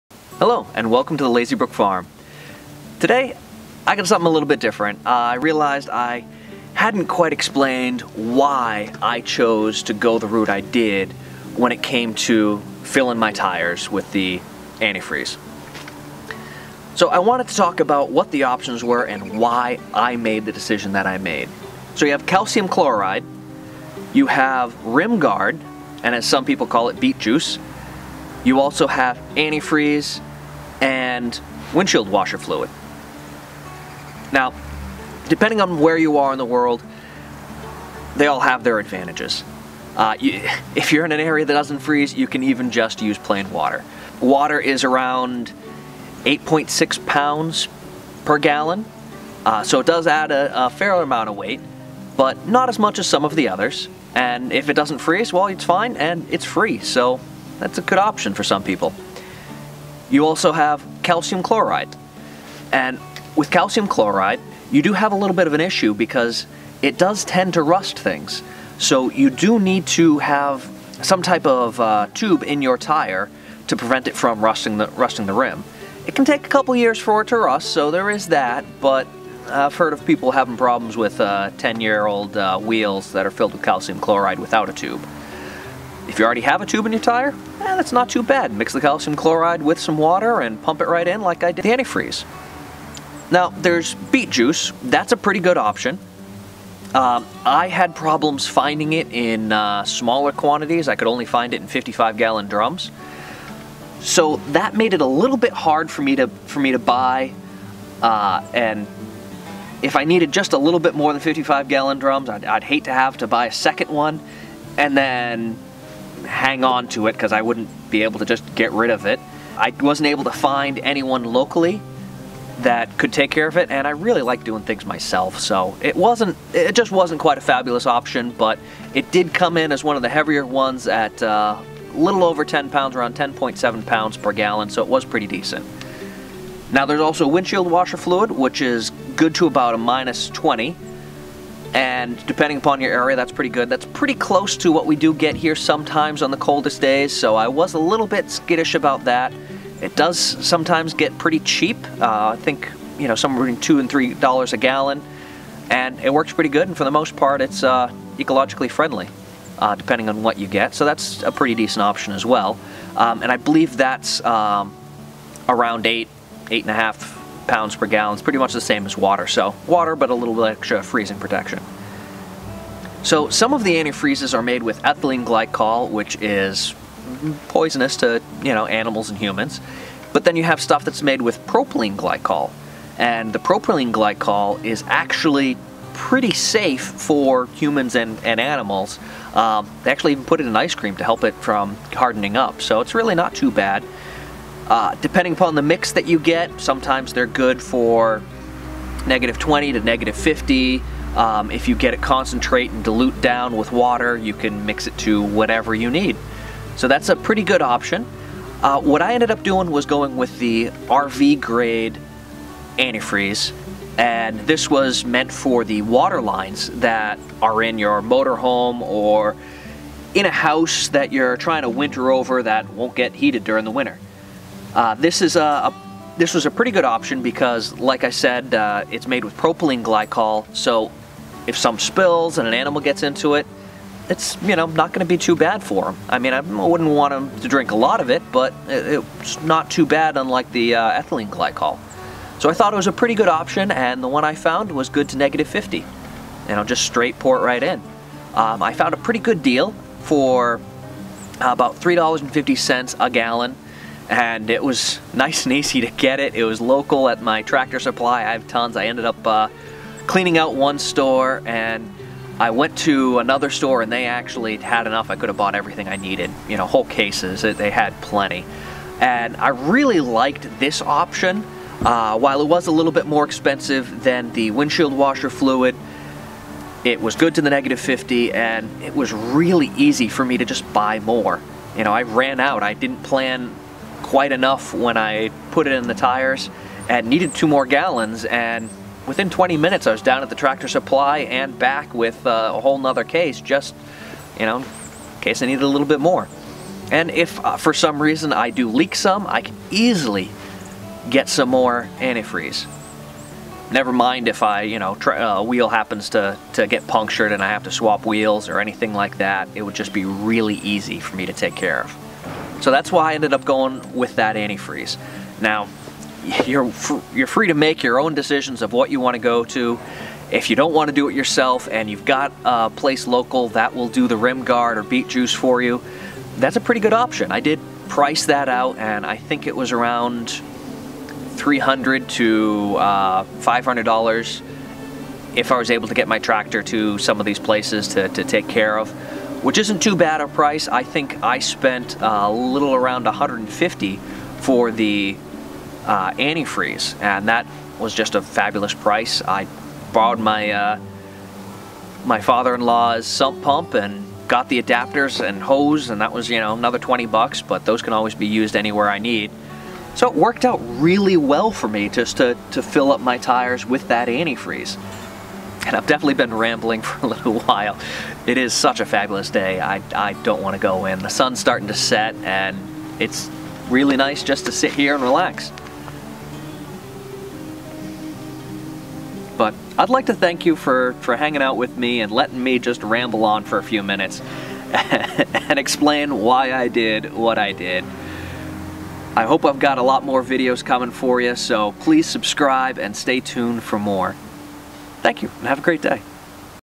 Why i chose - Tire Liquid Ballast
I wanted to talk about the Options I researched, and why I chose antifreeze as a liquid ballast for my tractor tires.

Main Camera 1
Main Camera 2
Action Camera
Studio Mic